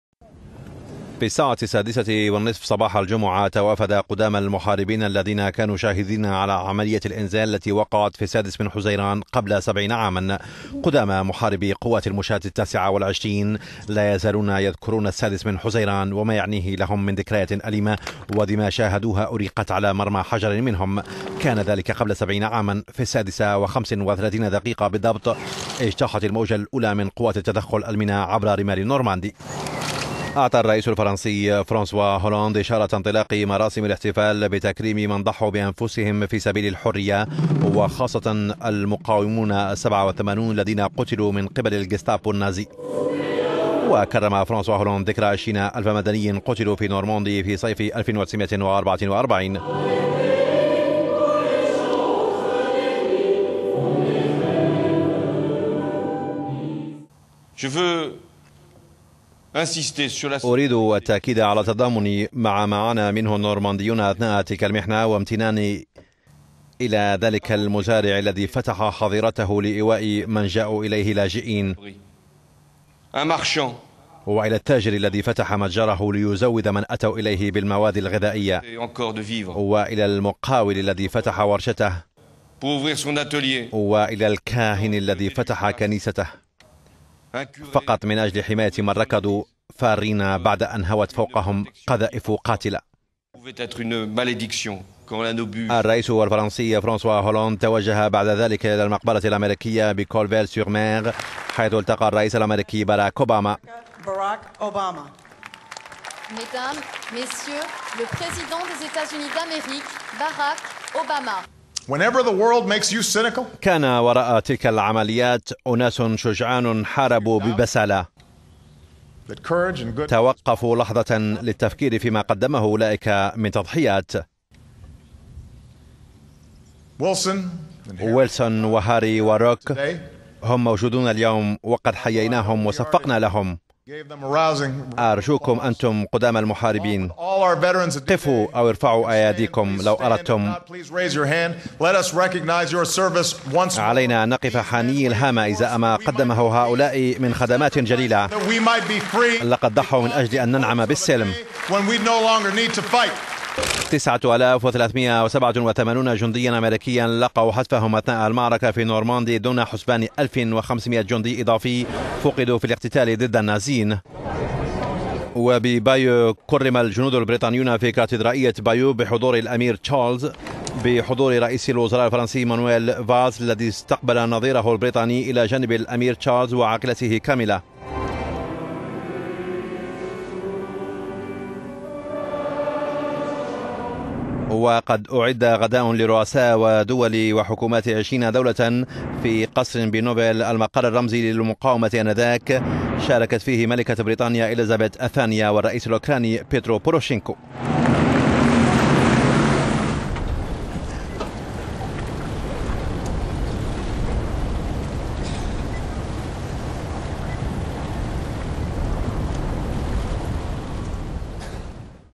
احتفالات بالذكرى السبعين لإنزال الحلفاء في النورماندي بحضور باراك أوباما
في الساعة السادسة و النصف صباح الجمعة، توافد قدامى المحاربين الذين كانوا شاهدين على عملية الإنزال التي وقت في السادس من حزيران/يونيو من العام1944، قدامى محاربي قوات المشاة التاسعة  و العشرين لا يزالون يذكرون السادس من حزيران و ما يعنيه لهم، من ذكريات أليمة  و دماء شاهدوها، أريقت على مرمى حجر منهم. كان ذلك قبل سبعين عاما، في السادسة و خمس و ثلاثين  دقيقة بالضبط،اجتاحت الموجة الأولى من قوات التدخل، الميناء، عبر رمال نورماندي.
أعطى الرئيس الفرنسي إشارة انطلاق مراسم الاحتفالات  بتكريم من ضحوا  بانف...